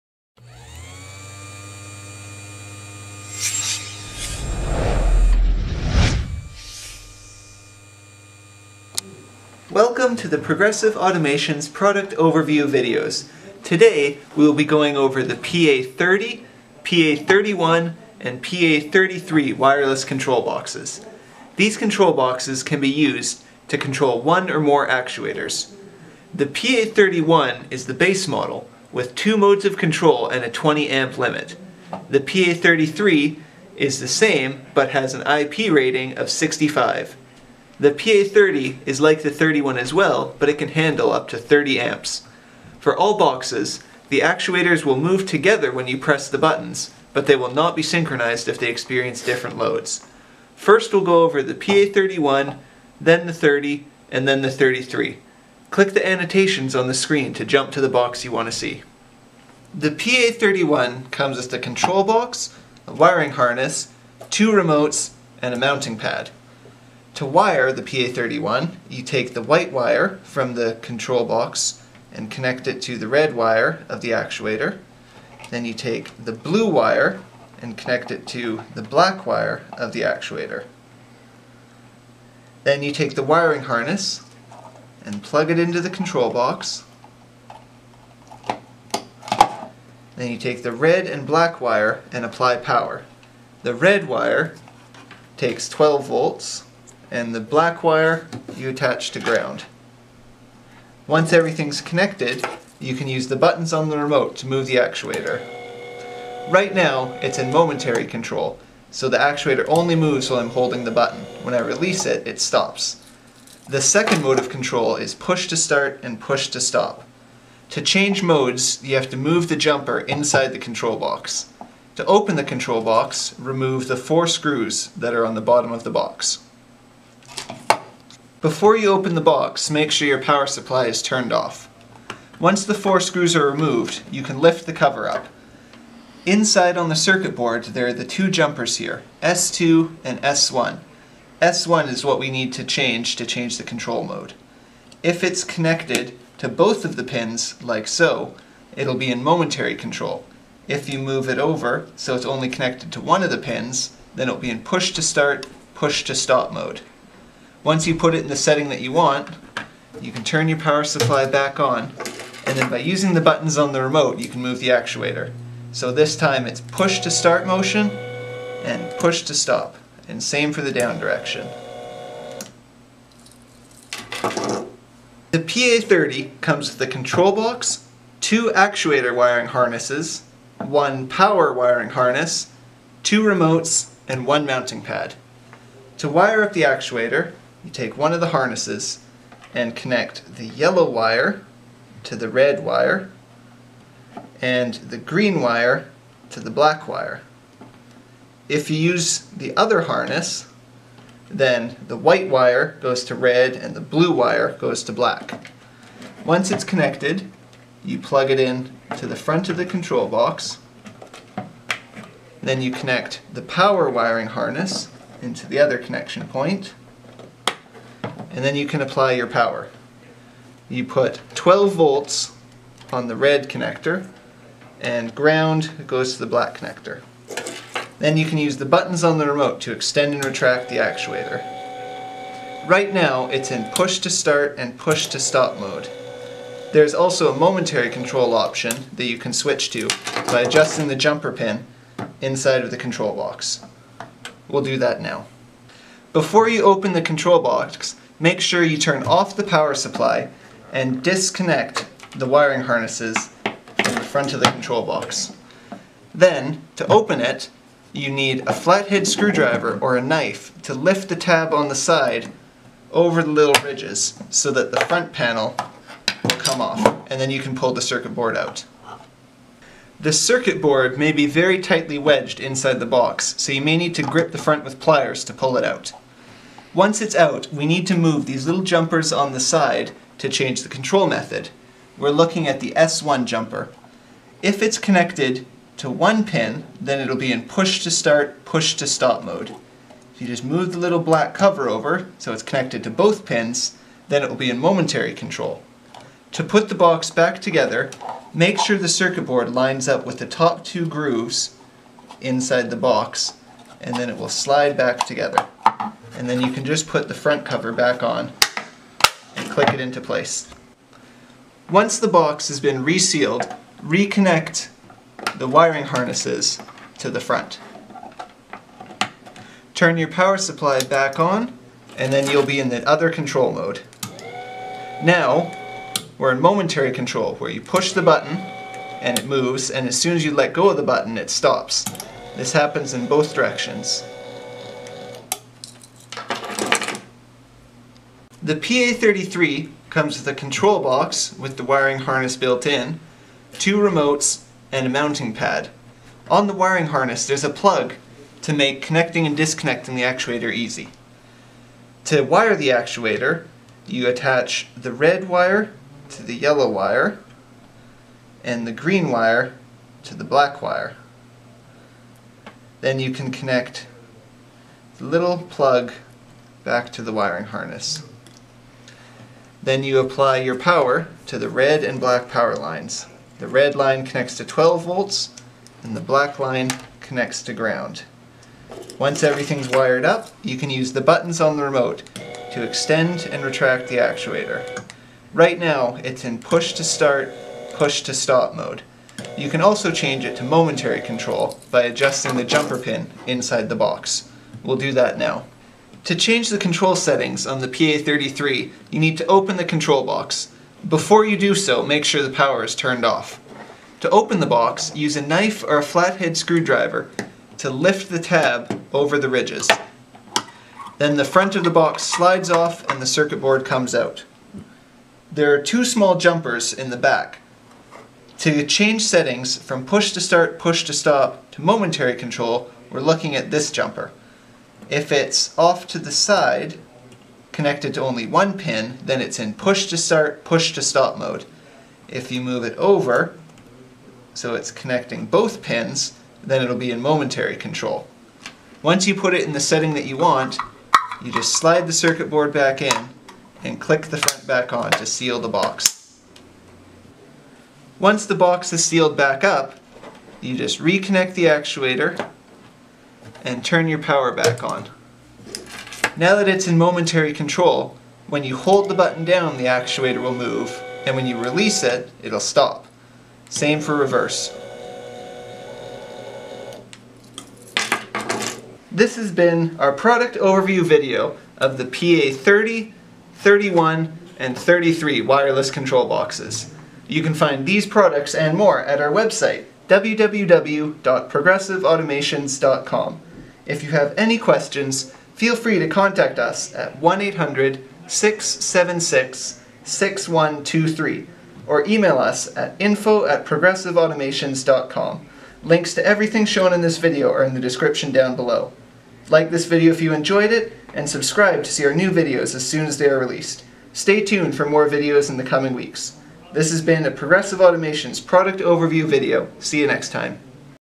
Remote Control Boxes PA-30, 31, 33 for Linear Actuators - Product Overview
In our latest product overview we go over how our PA-30 PA-31 and PA-33 Wireless Remote Control Boxes work and the features they offer. Our PA-33 also comes with an IP65 protection rating, meaning it is highly resistant to dust and strong jets of water, making it perfect for outdoor use.